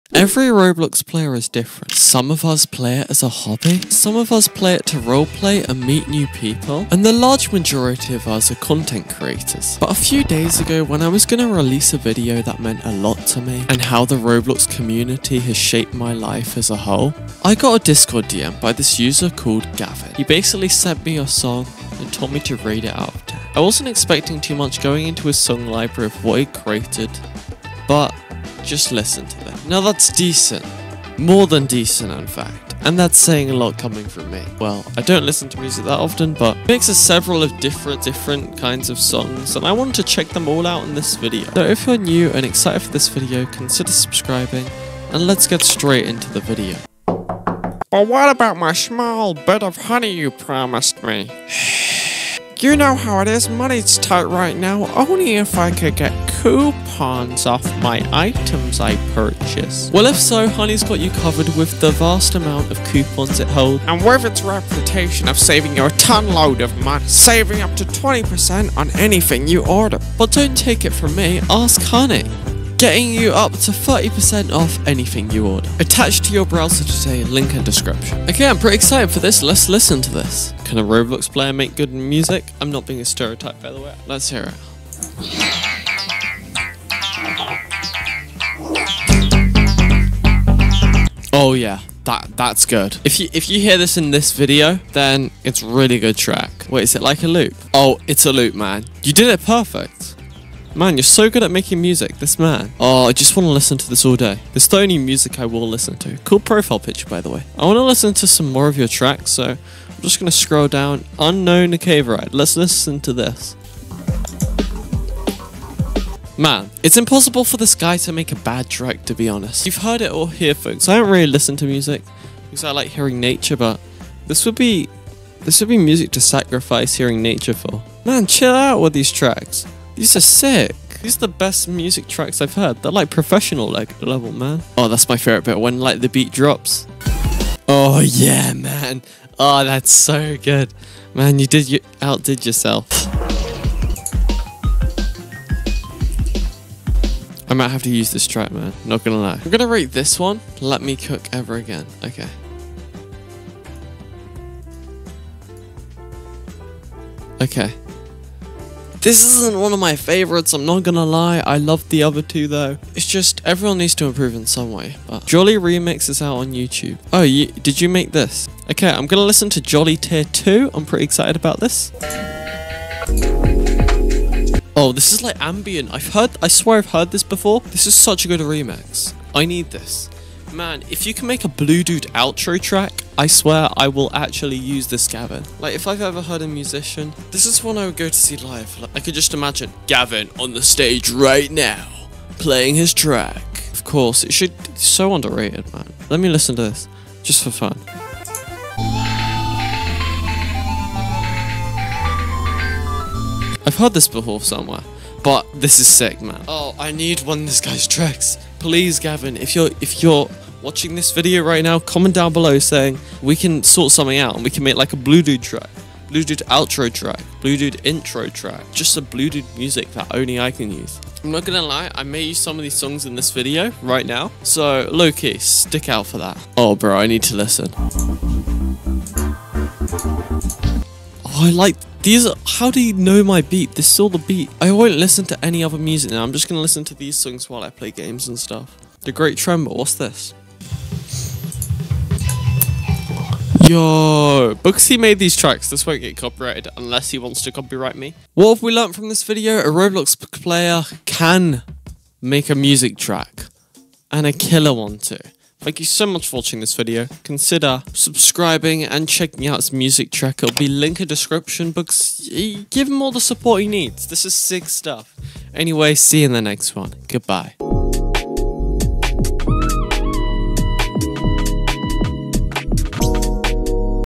Can Roblox YouTubers Make Music?!…
Can Roblox YouTubers make music, well today in this video I endeavor into one of my fans music library of songs he has created and is his hobby and they are brilliant it just shows anyone can make something meaningful or impactful just like these videos I like to feel genuine while most YouTubers overact put on this ridiculous act nowadays I stay true to myself and I know people may say you don’t sound excited that’s because it’s me

for Music go Sub to him